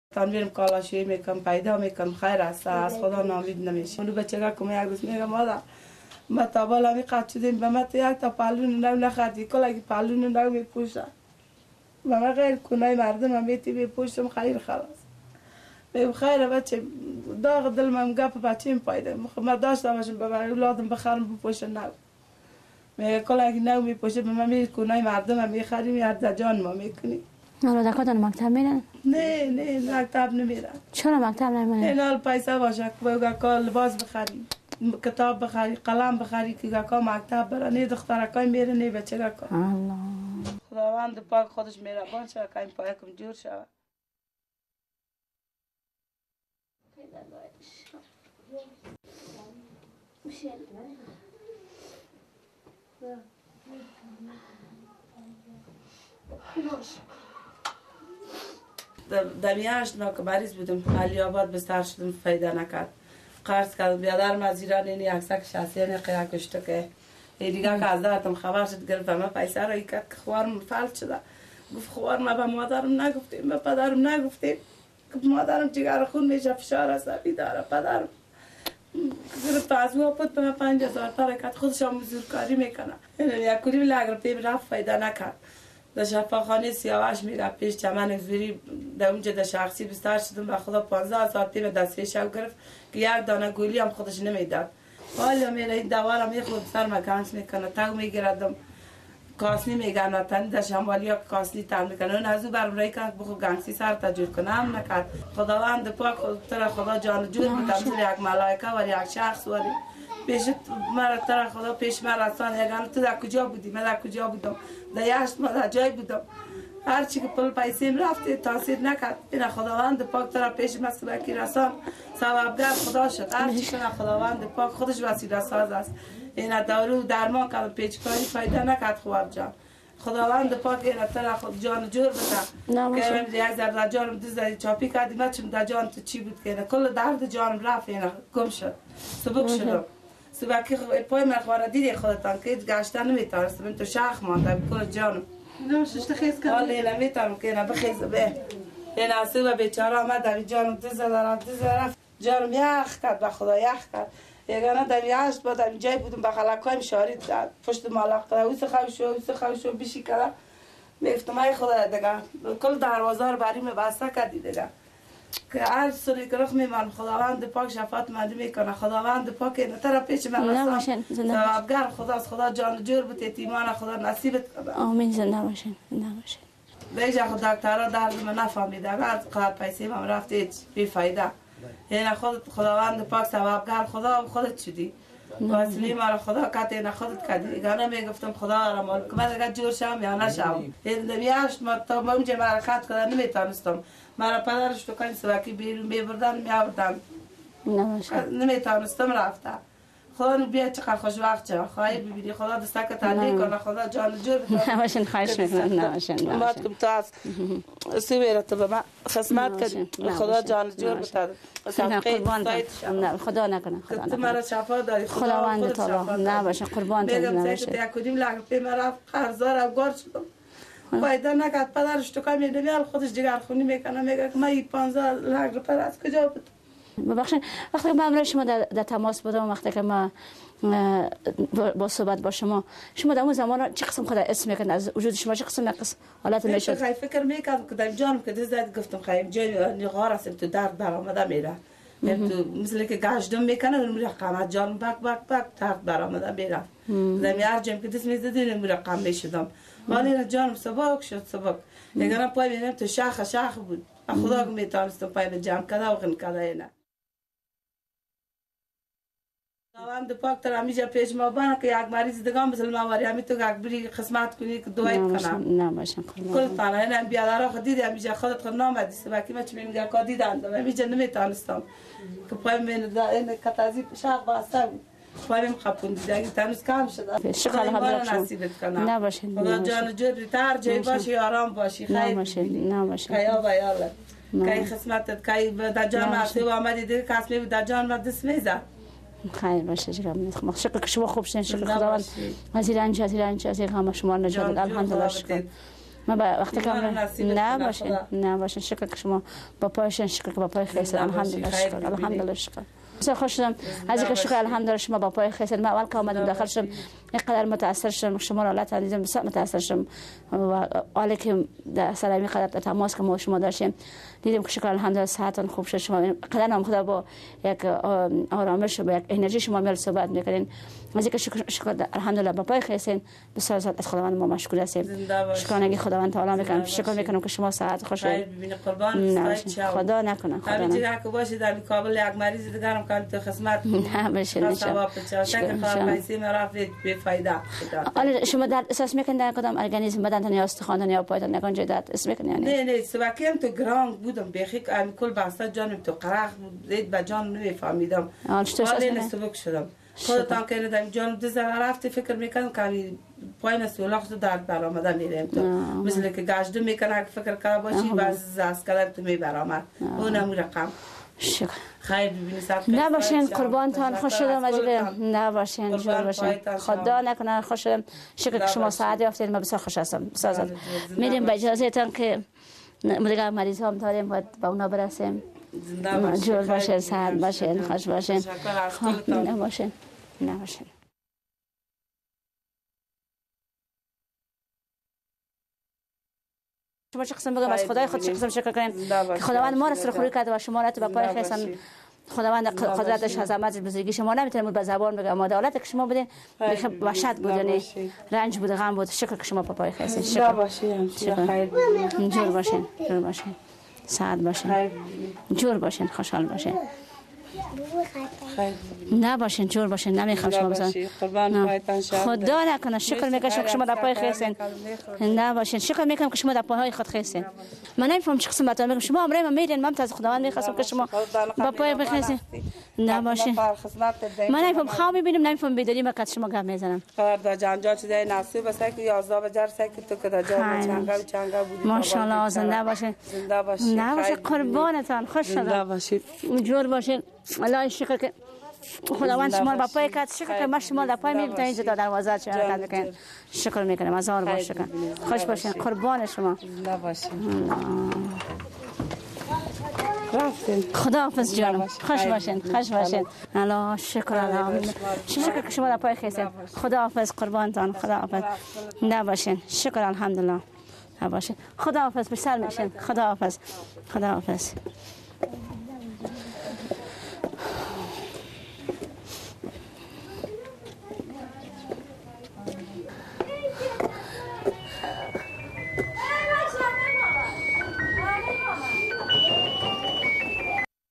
jamila nirobakhsh part 2
The journey of  Jamila Nirobakhsh in Afghanistan